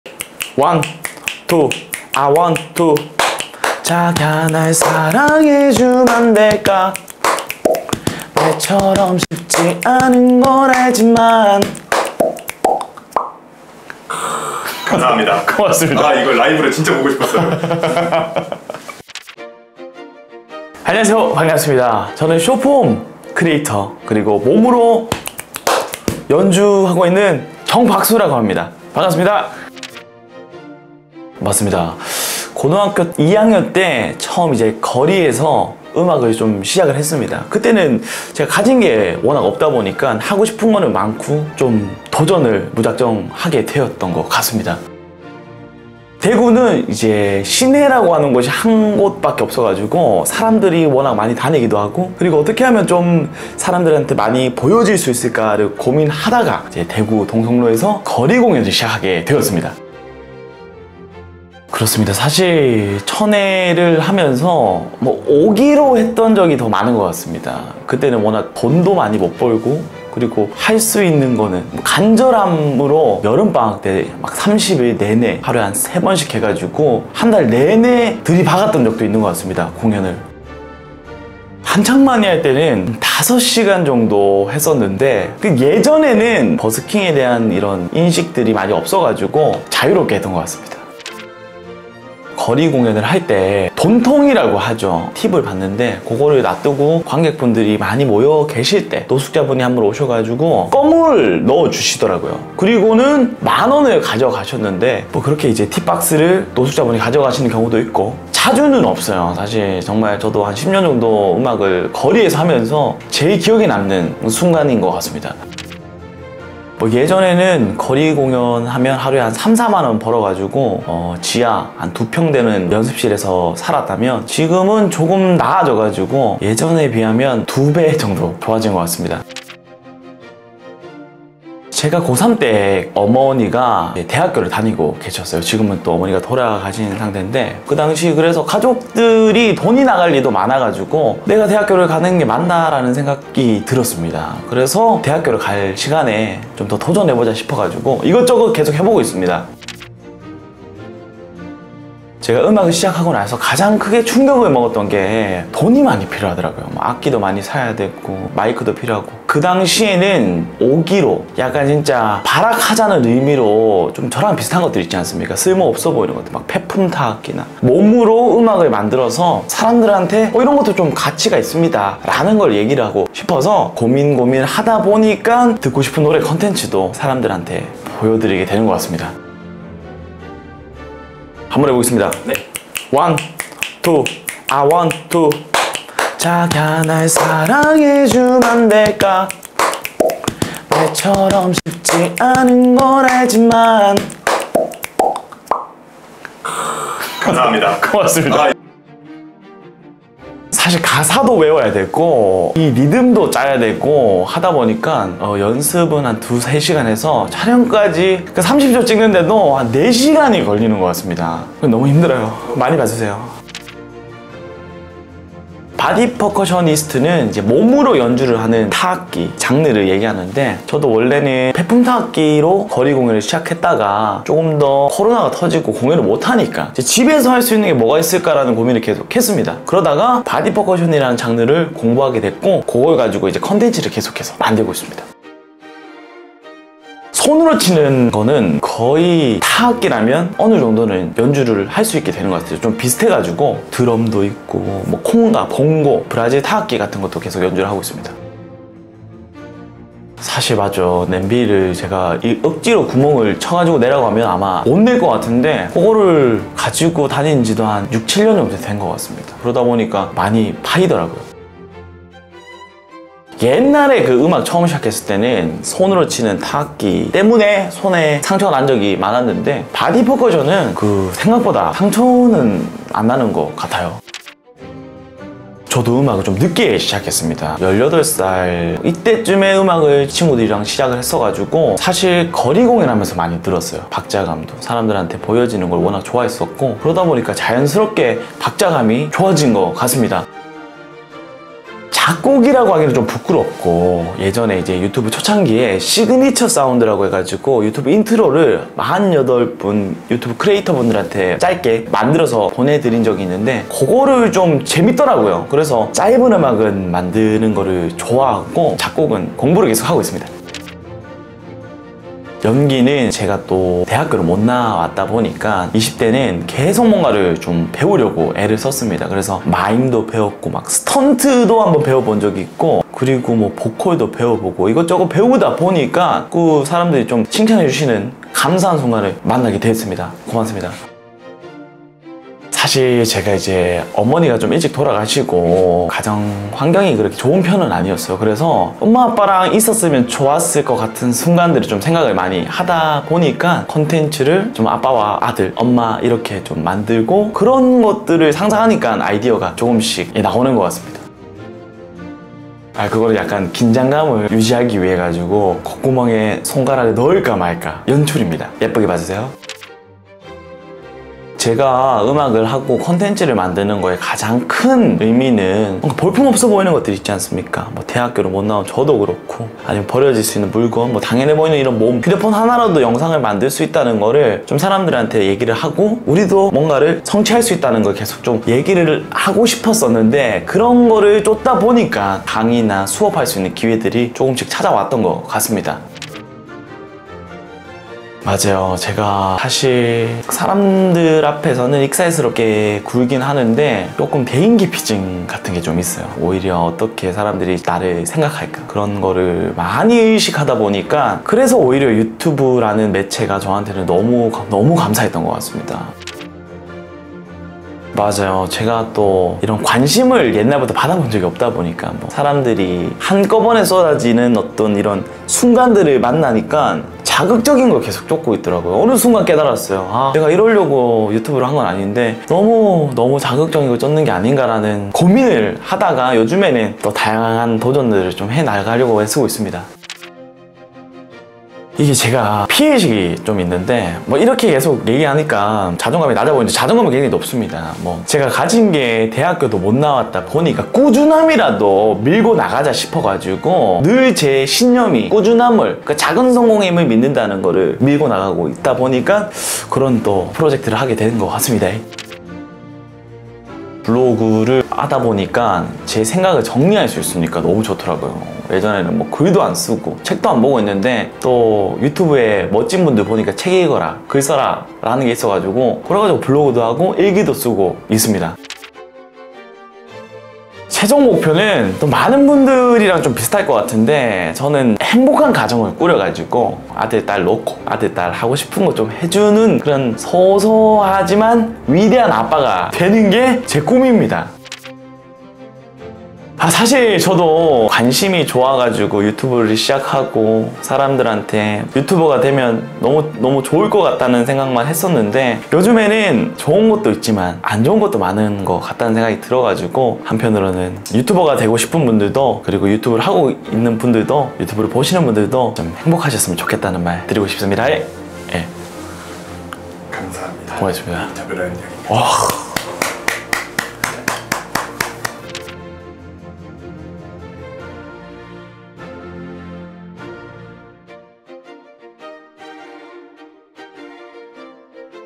대한민국 박수 원탑 정박수, 어머니가 돌아가신 후 대학을 가기도 힘들었던 삶ㅣ 하루 수입 3만원에서 현재는 XX배 높아짐
정박수, 당신이 몰랐던 16가지 사실ㅣ아빠 주머니에 5만원이 사라졌어...

0. 자기소개
1. 길거리 공연만 1천회
2. 노숙자
3. 수입
4. 대학 등록금
5. 듣고 싶은 노래 있어?
6. 바디 퍼커셔니스트
7. 박자감
8. 1인 다역
9. 박자감
10. 교육활동
11. 유튜브 및 틱톡시작
12. 조회 수와 좋아요
13. 1일 1영상
14. 블로그 운영
15. 최종 목표
16. 하고 싶은 말

※유튜브 공유 기능 외 무단 업로드 금지합니다.

세상의 모든 인물, 인물백과사전입니다.
더 많은 인물에 대해 알고 싶다면,
인물백과를 구독하고 받아보세요!